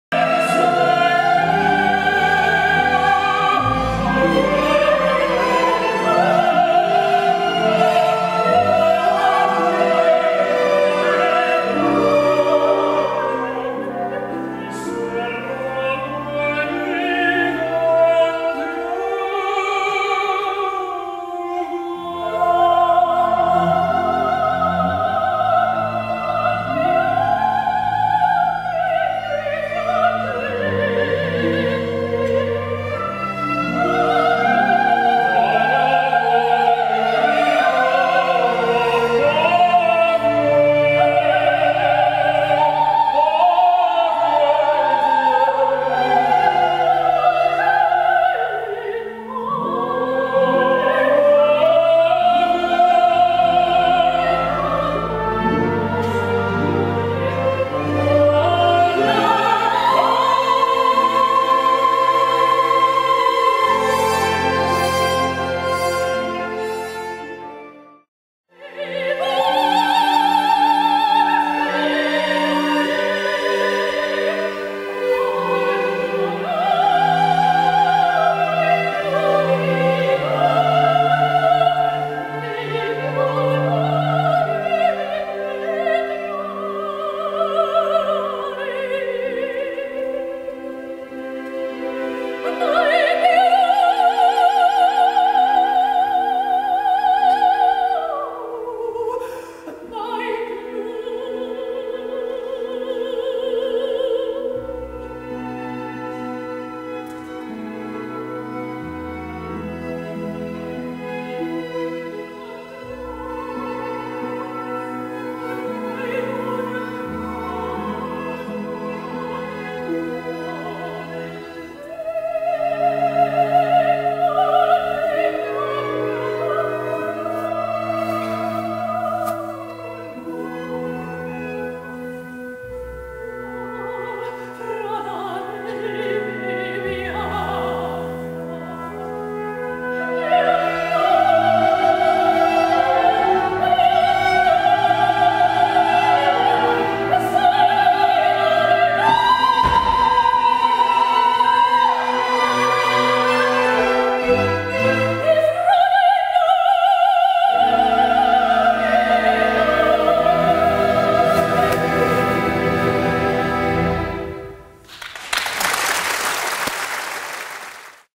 CALLAS - The Life & Music of Maria Callas. Written & Directed by Niall Morris
Stunning new show about the life of Opera Legend Maria Callas.
Drawing extraordinary parallels between her tragic life & the operatic roles she played.

International acclaimed tenor James Edwards as ONASSIS
Writer / Director: Niall Morris